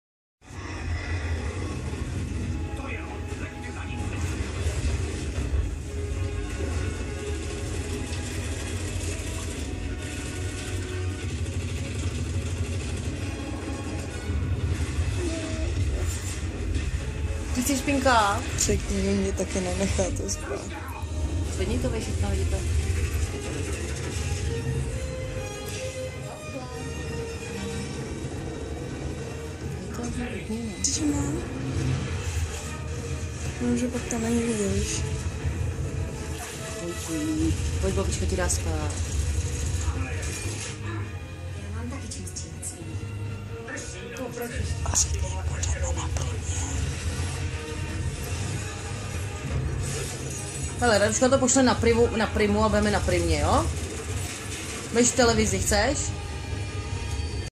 Někdo usnul
moc ho vysílilo koukání se na Avatara, bylo to komické. - Autor: Raduška Urbánkojc